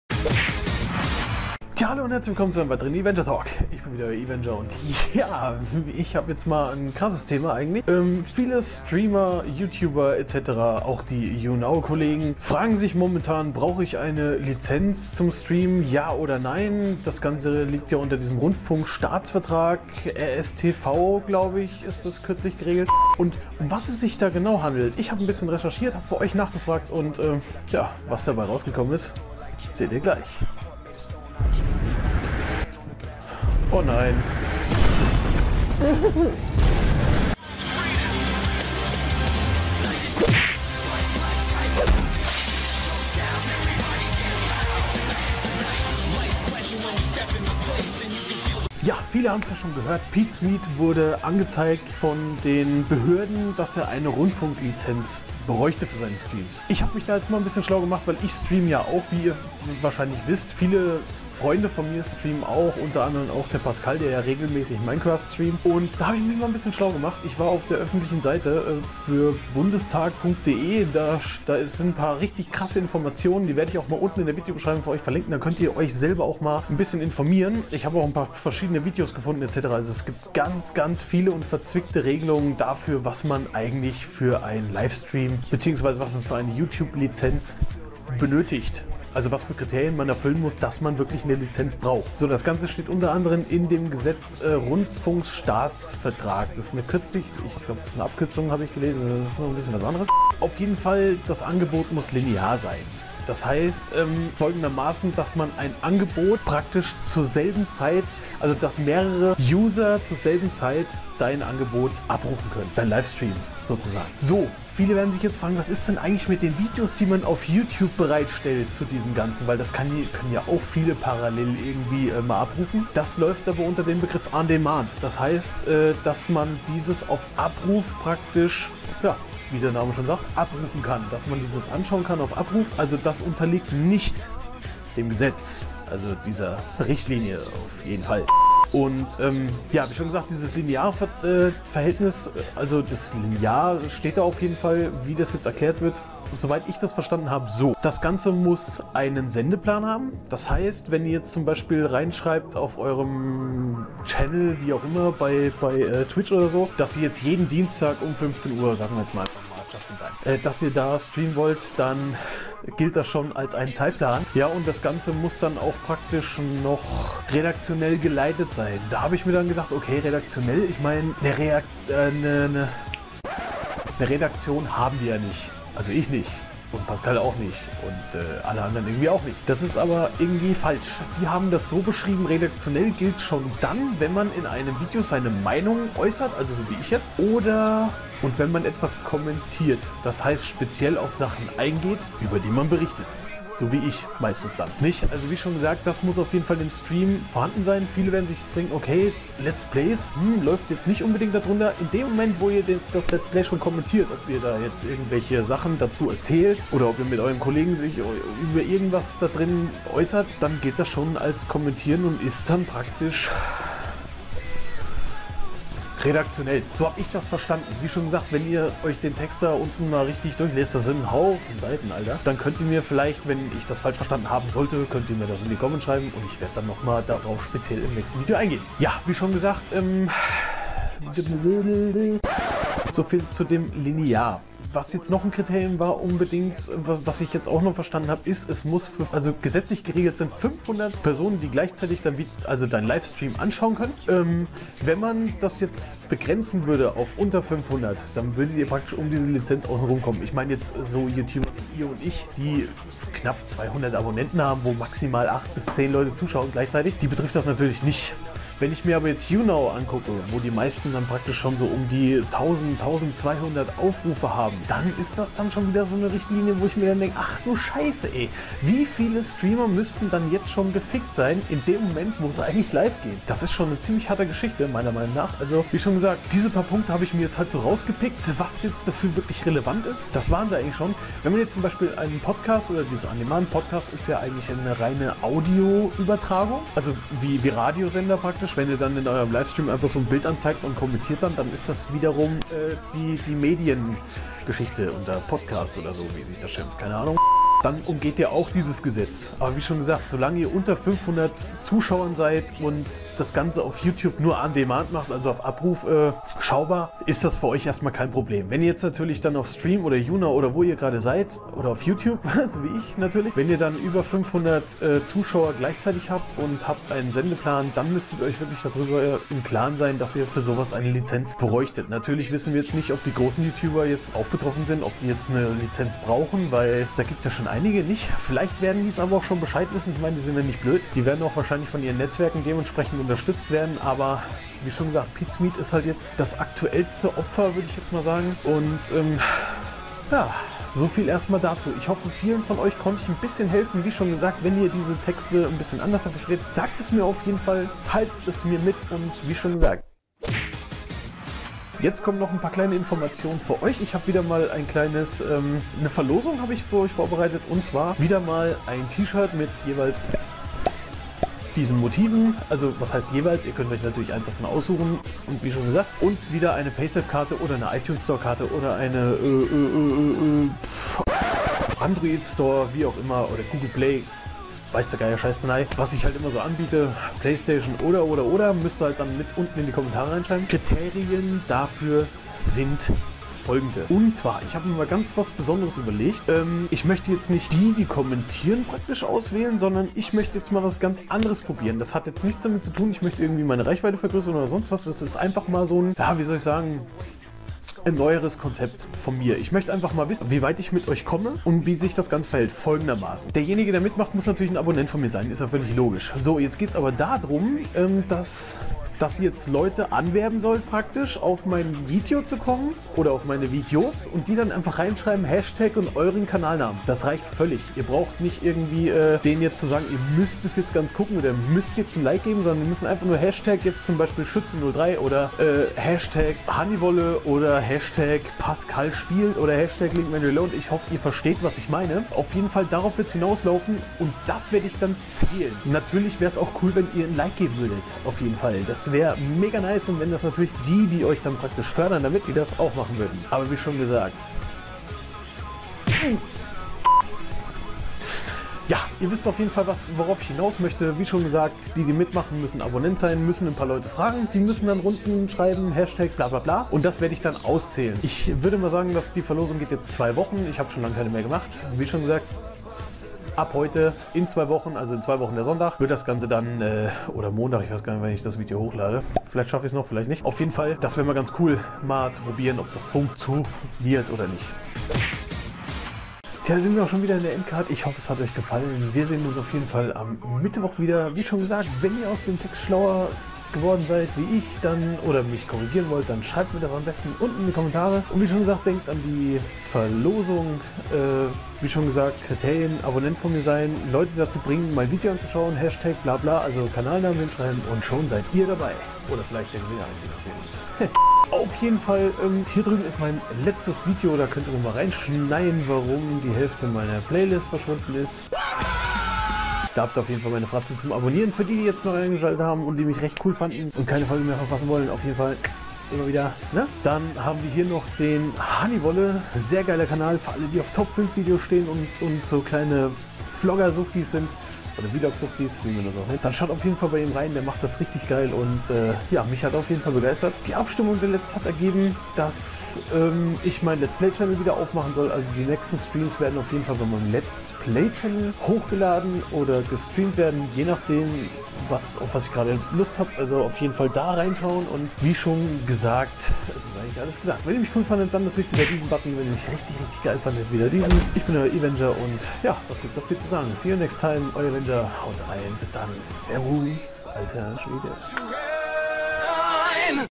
Verlosung/Rundfunklizenz? Welche Kriterien muss man erfüllen?
Ich habe etwas recherchiert und bin zu folgendem Ergebnis gekommen!
Muss jeder Streamer jetzt eine Rundfunklizenz haben??